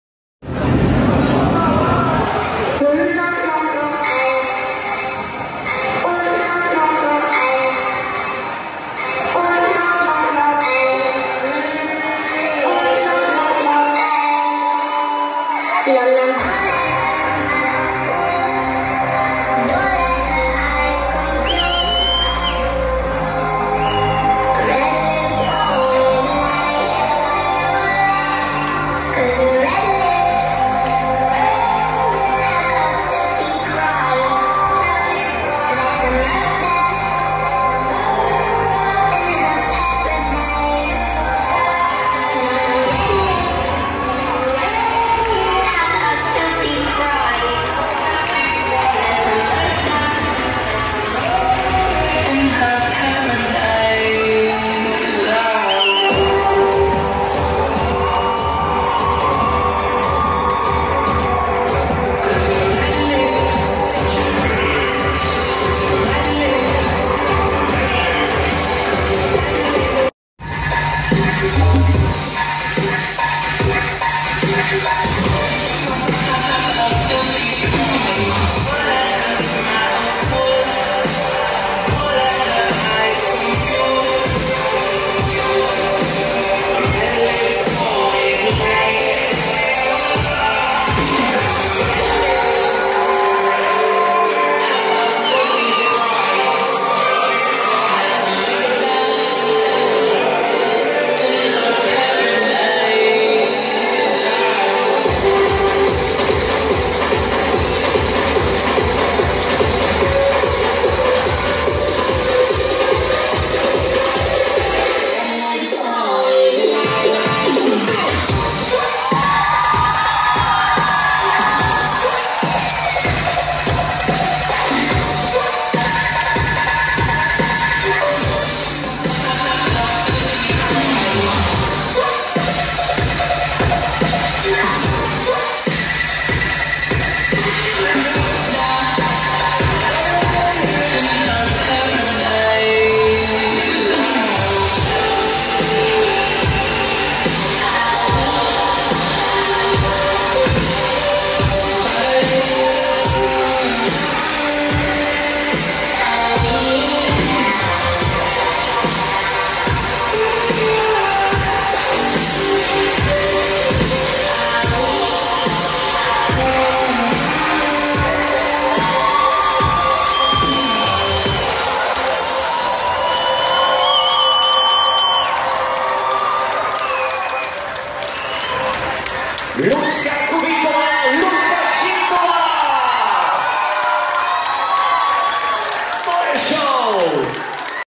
Pole dance show doubles - Lucie Šimková & Lucie Kubiková
Záznam z vystoupení na maturitním plese na téma "Chaos a pořádek"
Lucie Šimkové a Lucie Kubíkové